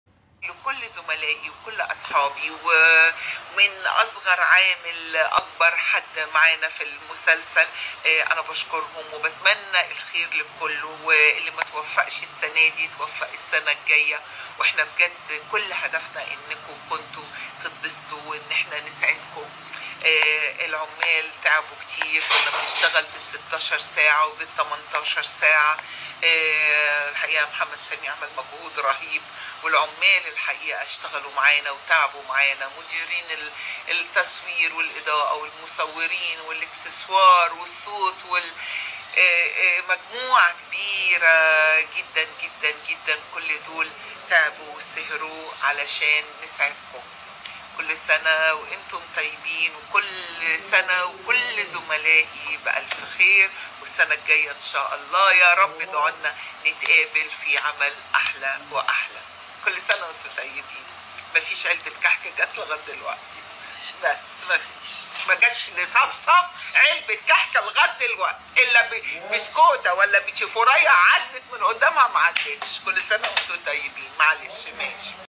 الفنانة هالة صدقي بعد نجاحها في مسلسل جعفر العمدة توجه رسالة؟؟
رسالة الملكة صفصف لكل المشاركين في مسلسل جعفر العمدة قوني كل هدفنا هو إسعادكم